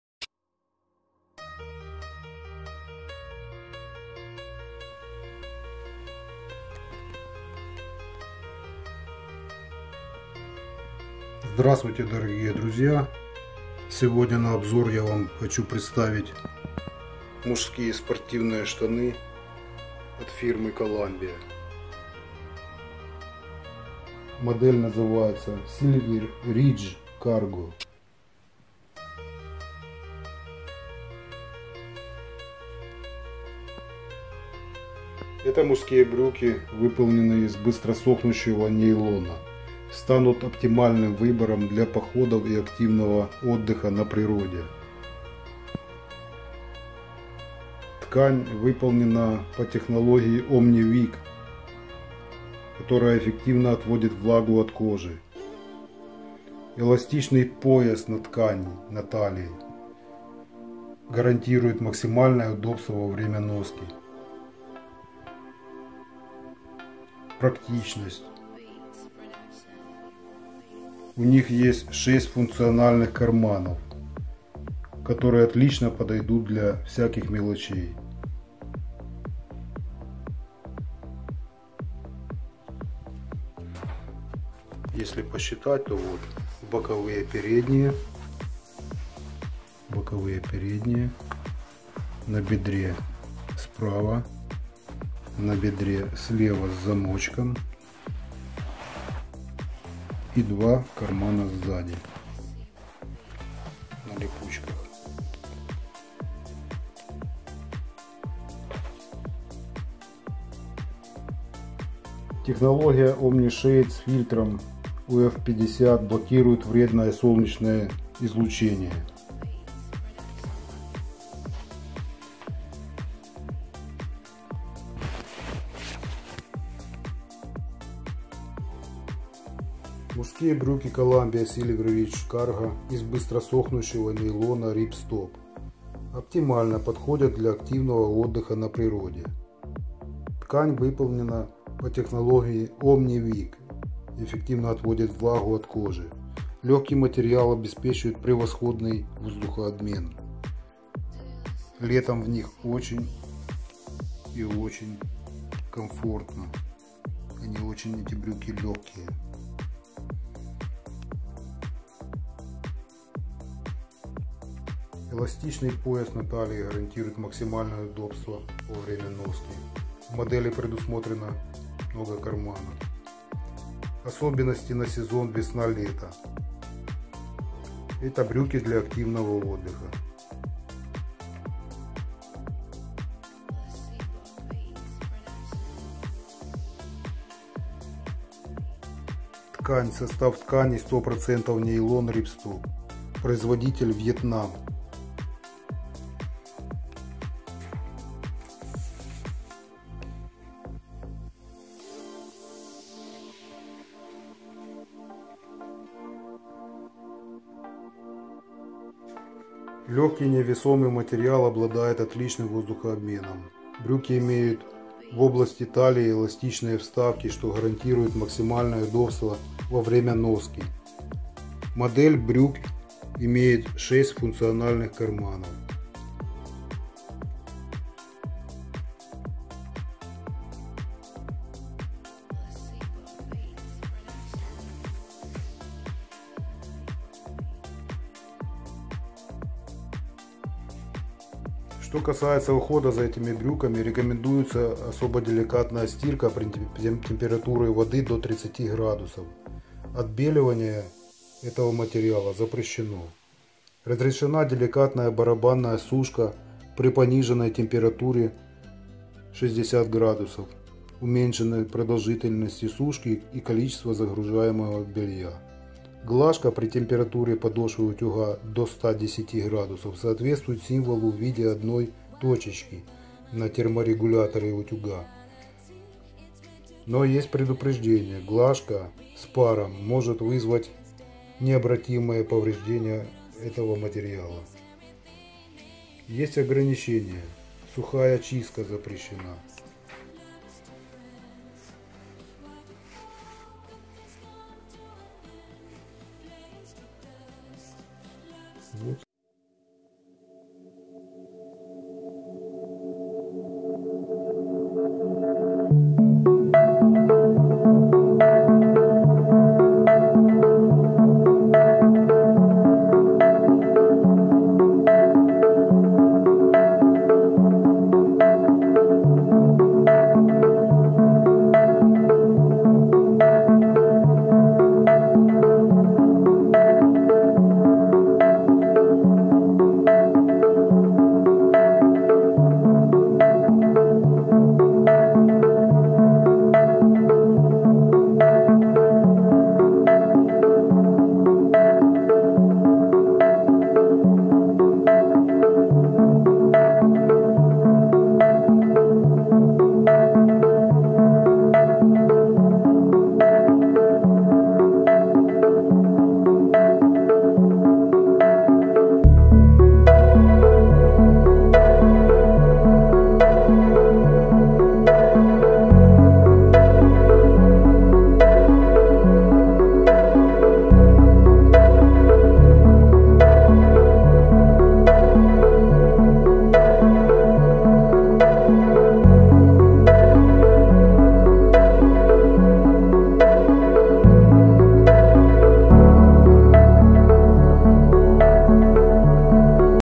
Обзор спортивных брюк Columbia Silver Ridge Cargo
Мужские спортивные брюки от Columbia, модель Silver Ridge Cargo, цвет синий, очень удобны при носке в жаркую погоду. Лучше иметь одни такие, чем 10 пар джинсов.